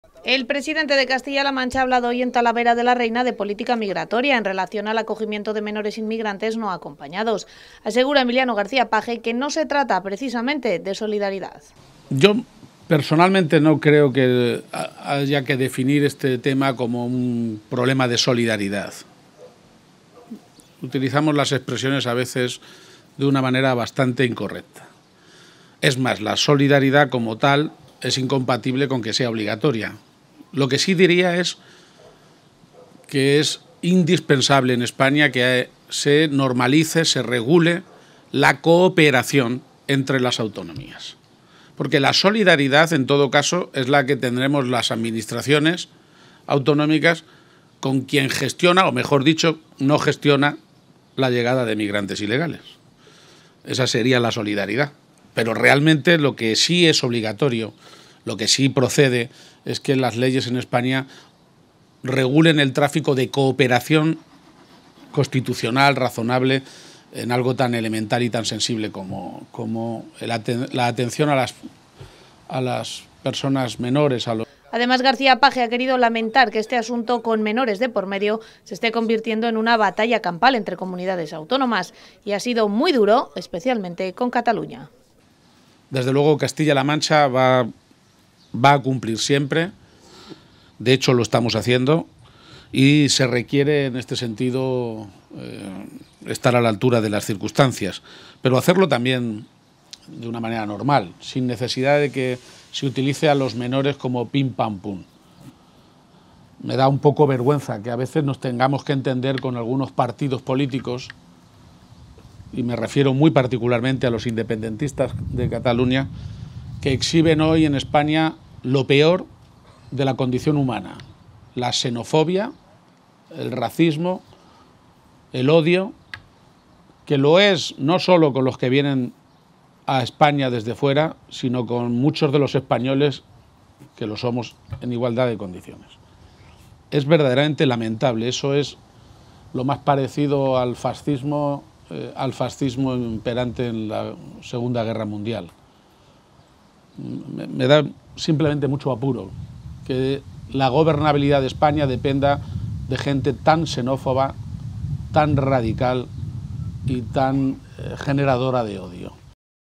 Page lamenta que el acogimiento de menores sea una batalla campal
El presidente de Castilla La Mancha ha arremetido duramente contra Cataluña asegurando que no se puede poner encima de la mesa una  posición tan xenófoba para con inmigrantes y resto de españoles y ser socio de gobierno.